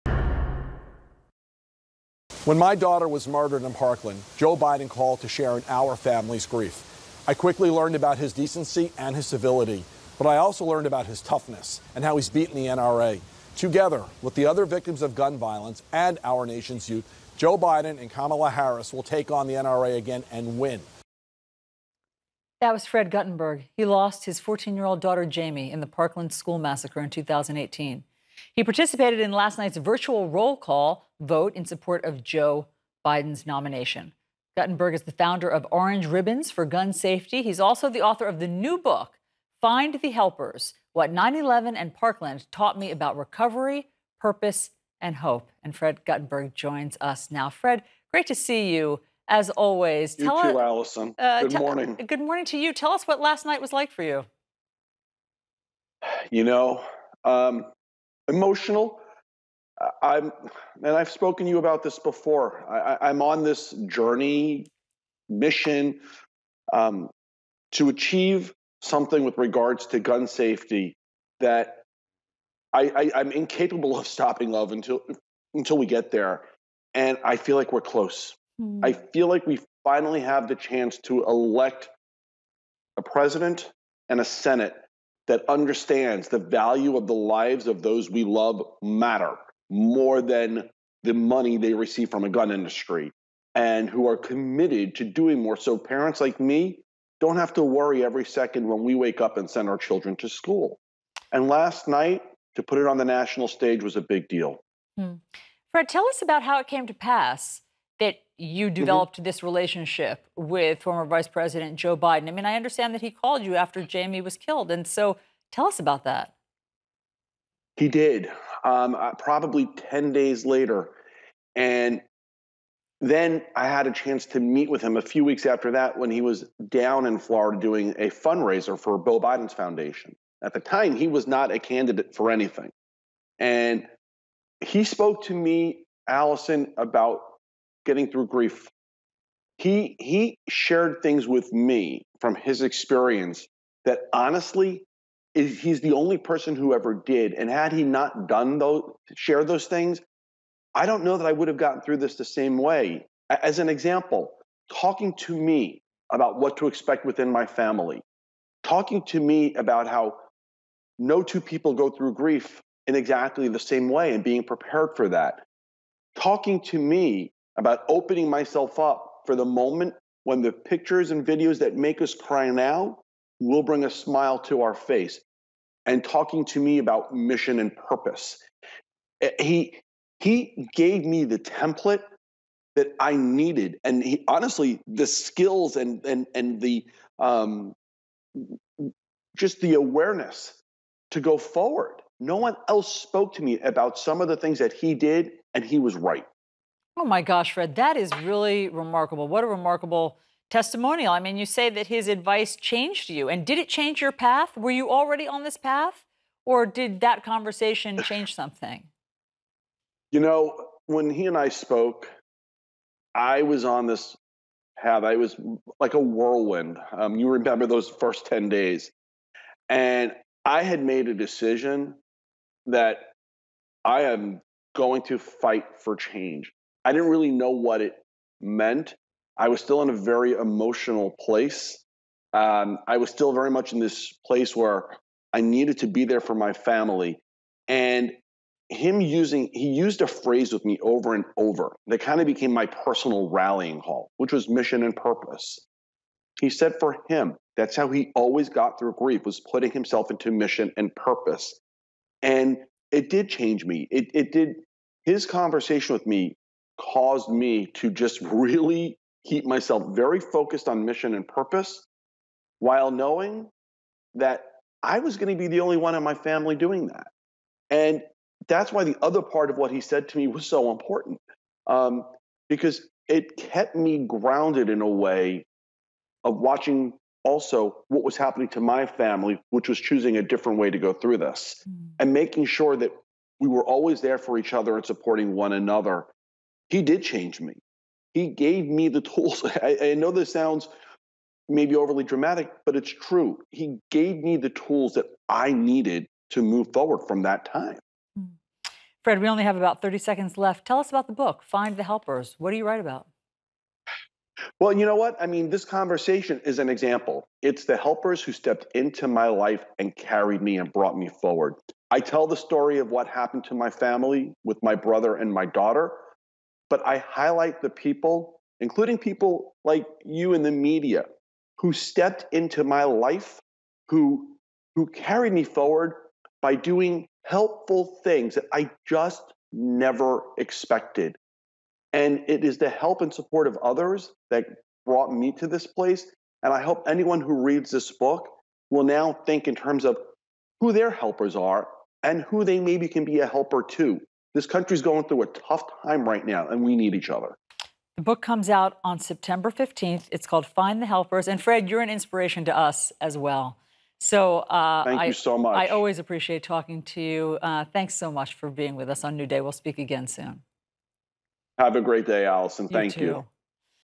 Father of a Parkland student speaks on Joe Biden
I always appreciate hearing from Fred Guttenberg, father of Jamie, a fourteen year old killed in the Parkland massacre. He previewed his book for us this morning and explained what Joe Biden taught him about grieving and how to go on with life.